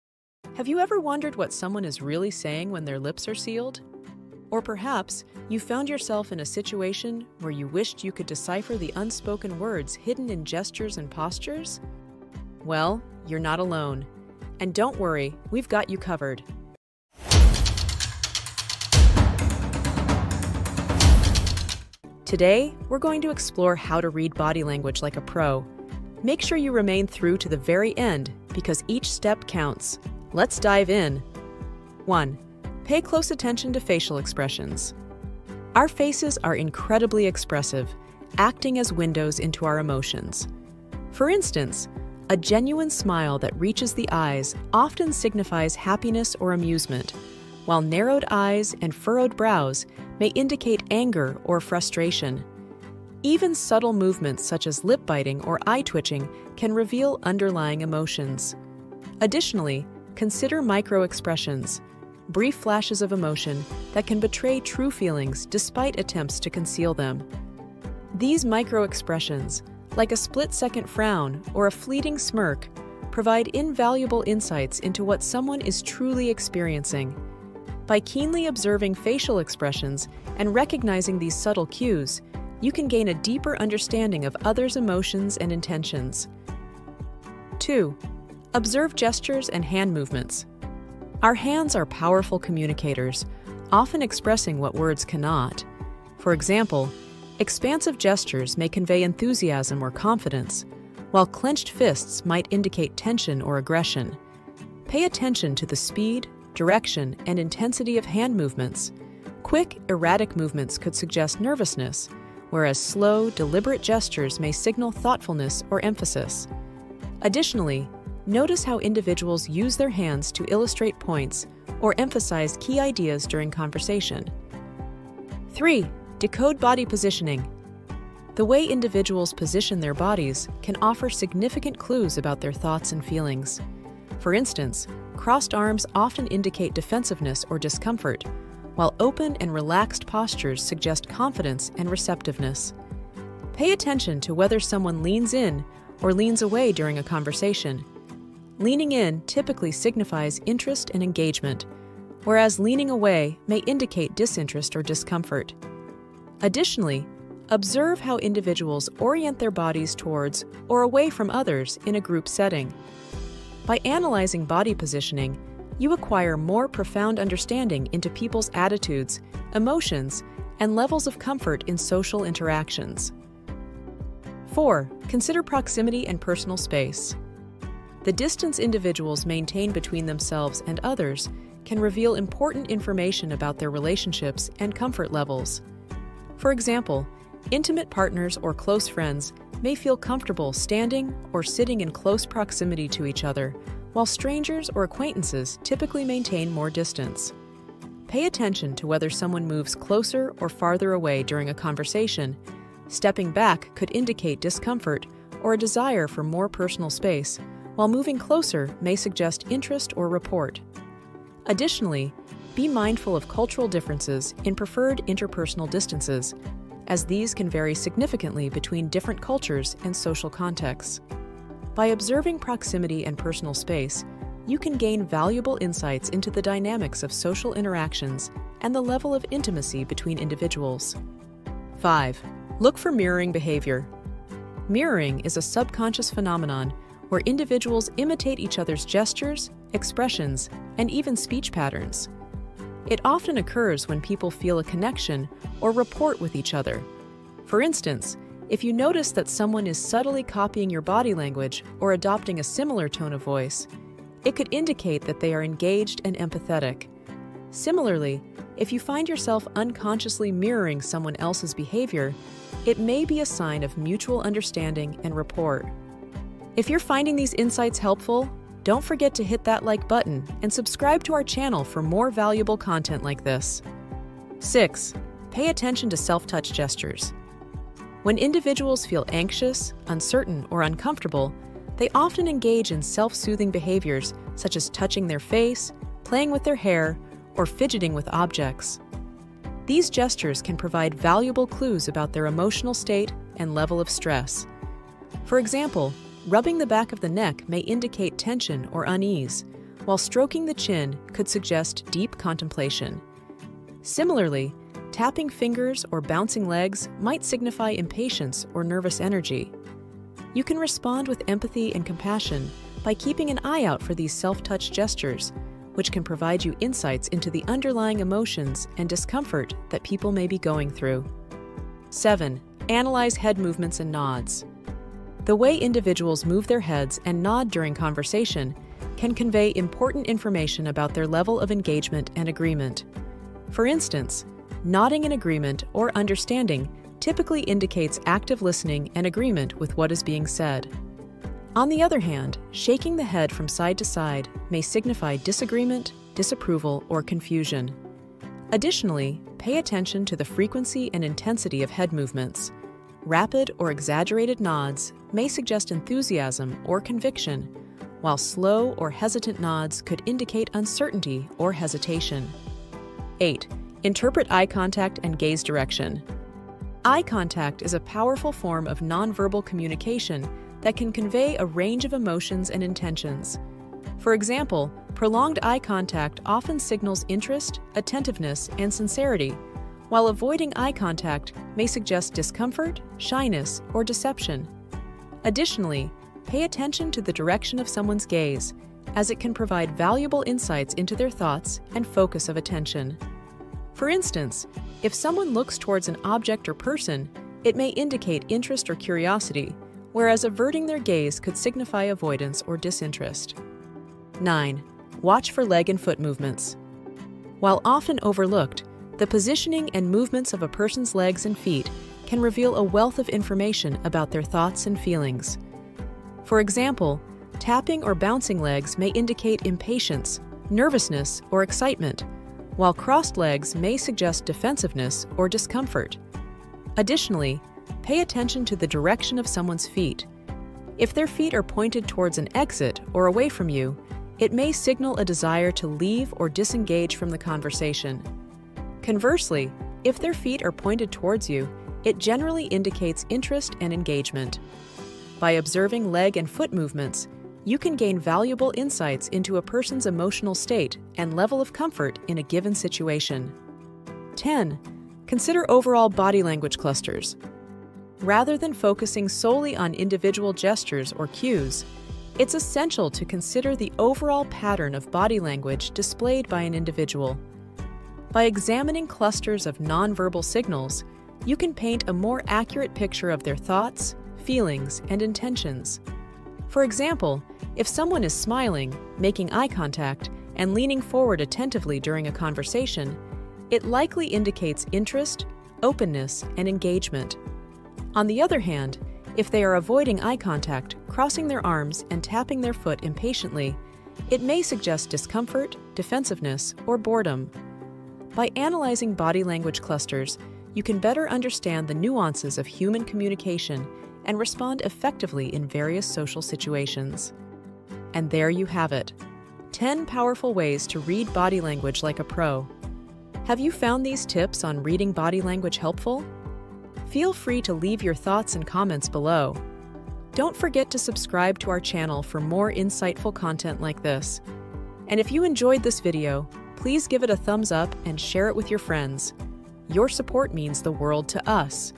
10 Ways to Read Body Language Like a PRO
How to Read Body Language Like a PRO

Delve into the intricate world of human communication, where facial expressions and psychological insights are the keys to unlocking understanding. Explore how mastering body language can unveil romantic interest or reveal someone's true character. Learn to project confidence through astute observation and psychological strategies, gaining instant insights into others' personalities. Understand the profound impact of reading people accurately on effective communication, from subtle cues to deeper psychological undercurrents. Embrace the challenge of decoding human behavior, honing your skills for enhanced interpersonal connections and a deeper understanding of the human psyche.

🔥Learn More about **PSYCHOLOGY**🔥

Free **Success Planner** PDF:
📚 Boost Your Productivity for FREE! 📚

Credit:
Channel art graphics & thumbnails created with the help of Canva and pexels.